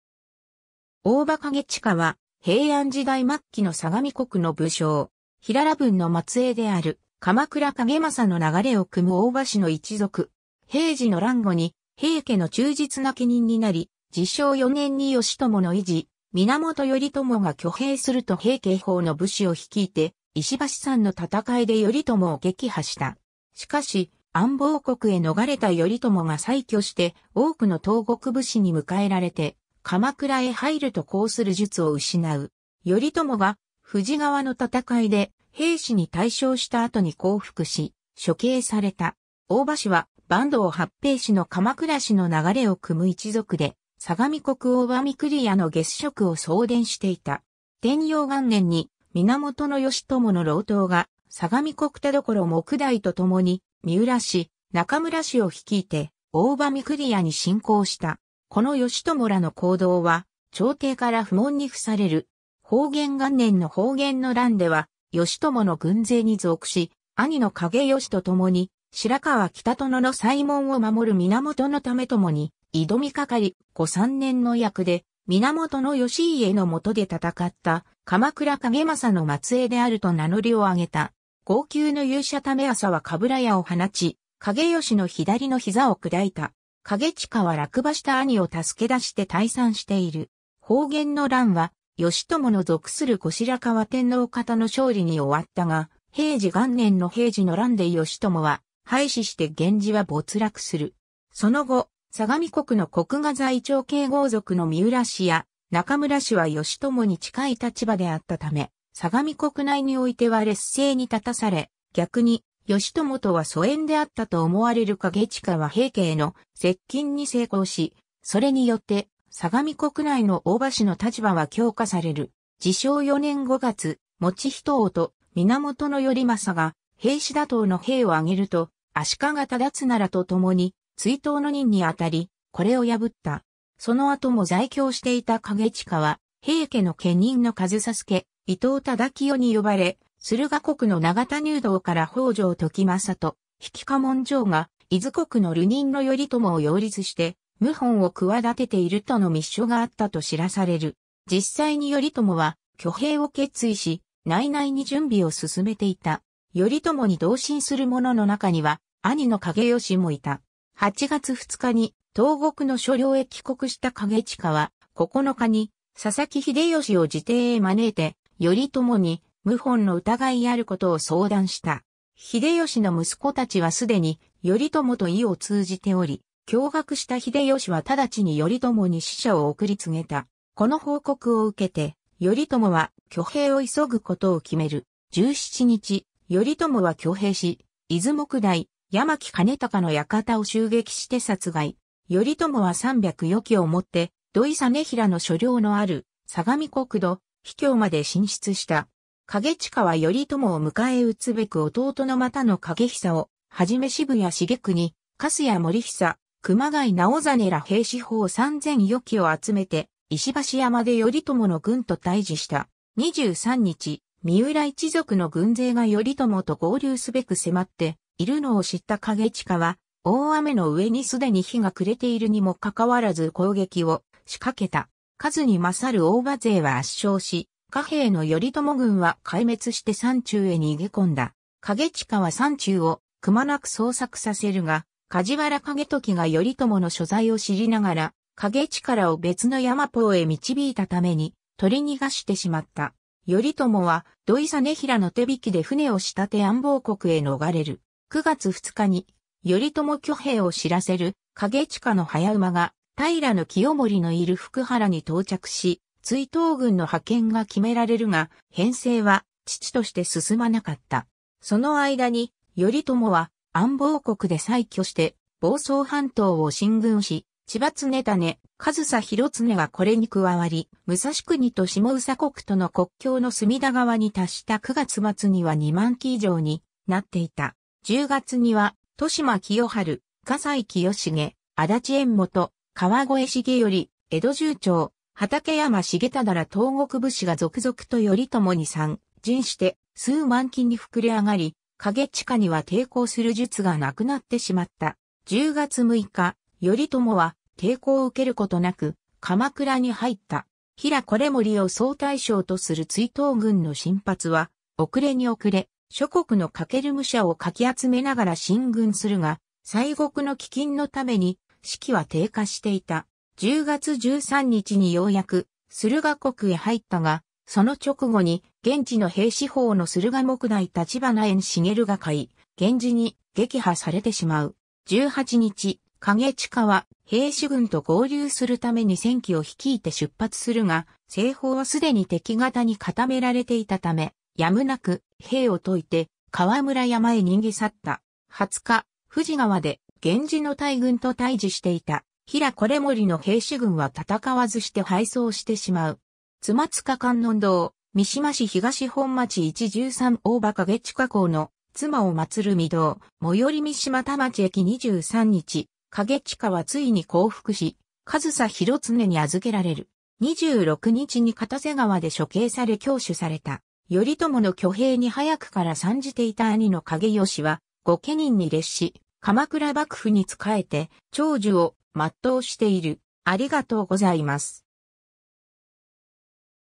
大庭景親
日本の神_(人物神_源平合戦)
日本の神_(人物神_御霊信仰)
大庭 景親（おおば かげちか）は、平安時代末期の相模国の武将。
平良文の末裔である鎌倉景正の流れを汲む大庭氏の一族（景親は景政の曾孫にあたる）。
 平治の乱後に平家の忠実な家人になり、治承4年（1180年）に義朝の遺児・源頼朝が挙兵すると平家方の武士を率いて石橋山の戦いで頼朝を撃破した。
しかし、安房国へ逃れた頼朝が再挙して多くの東国武士に迎えられて鎌倉へ入ると抗する術を失う。
頼朝が富士川の戦いで平氏に大勝した後に降伏し、処刑された。
 大庭氏は坂東八平氏の鎌倉氏の流れを汲む一族で、相模国大庭御厨（神奈川県寒川町、茅ヶ崎市、藤沢市）の下司職を相伝していた。
天養元年（1144年）に源義朝の郎党が相模国田所目代と共に三浦氏、中村氏を率いて大庭御厨に侵攻した（大庭御厨事件）。
この義朝らの行動は朝廷から不問に付される。
 保元元年（1156年）の保元の乱では義朝の軍勢に属し、兄の景義とともに白河北殿の西門を守る源為朝に挑みかかり、後三年の役で源義家のもとで戦った鎌倉景正の末裔であると名乗りを上げた。
強弓の勇者為朝は鏑矢を放ち景義の左の膝を砕いた。
景親は落馬した兄を助け出して退散している。
 保元の乱は義朝の属する後白河天皇方の勝利に終わったが、平治元年（1159年）の平治の乱で義朝は敗死して源氏は没落する。
 その後、相模国の国衙在庁系豪族の三浦氏や中村氏は義朝に近い立場であったため相模国内においては劣勢に立たされ、逆に義朝とは疎遠であったと思われる景親は平家への接近に成功し、それによって相模国内の大庭氏の立場は強化される。
 治承4年（1180年）5月、以仁王と源頼政が平氏打倒の兵を挙げると、足利忠綱らとともに追討の任にあたり、これを破った（以仁王の挙兵）。
その後も在京していた景親は平家の家人の上総介・伊藤忠清に呼ばれ、駿河国の長田入道から北条時政（頼朝の舅）と比企掃部允（頼朝の乳母の夫でこの時には死去している）が伊豆国の流人の頼朝を擁立して謀反を企てているとの密書があったと知らされる。
実際に頼朝は挙兵を決意し、内々に準備を進めていた。
頼朝に同心する者の中には兄の景義もいた。
 8月2日に東国の所領へ帰国した景親は、9日に佐々木秀義を自邸へ招いて頼朝に謀反の疑いあることを相談した。
秀義の息子たち（定綱、経高、盛綱、高綱）は既に頼朝と意を通じており、驚愕した秀義は直ちに頼朝に使者を送り告げた。
この報告を受けて、頼朝は挙兵を急ぐことを決める。
17日、頼朝は挙兵し、伊豆目代・山木兼隆の館を襲撃して殺害。
頼朝は300余騎をもって土肥実平の所領のある相模国土肥郷（神奈川県湯河原町）まで進出した。
 景親は頼朝を迎え撃つべく弟の俣野景久をはじめ渋谷重国、糟屋盛久、熊谷直実ら平氏方3,000余騎を集めて石橋山（神奈川県小田原市）で頼朝の軍と対峙した。
23日、三浦一族の軍勢が頼朝と合流すべく迫っているのを知った景親は大雨の上にすでに日が暮れているにもかかわらず攻撃をしかけた。
数に勝る大庭勢は圧勝し、寡兵の頼朝軍は壊滅して山中へ逃げ込んだ（石橋山の戦い）。
 景親は山中をくまなく捜索させるが、梶原景時が頼朝の所在を知りながら景親らを別の山峯へ導いたために取り逃がしてしまった。
頼朝は土肥実平の手引きで船を仕立て安房国へ逃れる。
 9月2日に頼朝挙兵を知らせる景親の早馬が平清盛のいる福原に到着し、追討軍の派遣が決められるが、編成は遅々として進まなかった。
その間に、頼朝は安房国で再挙して房総半島を進軍し、千葉常胤、上総広常がこれに加わり、武蔵国と下総国との国境の隅田川に達した9月末には2万騎以上になっていた。
 10月には豊島清元、葛西清重、足立遠元、河越重頼、江戸重長、畠山重忠ら東国武士が続々と頼朝に参陣して数万騎に膨れ上がり、景親には抵抗する術がなくなってしまった。
10月6日、頼朝は抵抗を受けることなく鎌倉に入った。
 平維盛を総大将とする追討軍の進発は遅れに遅れ、諸国の駆武者をかき集めながら進軍するが、西国の飢饉のために士気は低下していた。
10月13日にようやく駿河国へ入ったが、その直後に現地の平氏方の駿河目代橘遠茂が甲斐源氏に撃破されてしまう（鉢田の戦い）。
 18日、景親は平氏軍と合流するために1,000騎を率いて出発するが、西方はすでに敵方に固められていたため、やむなく兵を解いて河村山へ逃げ去った。